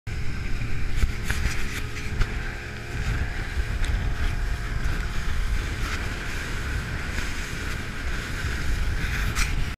Thorne SAC UBoat SM UC75
Heading out to dive the U-Boat UC-70 on board Major Tom - launch from Hornsea Marina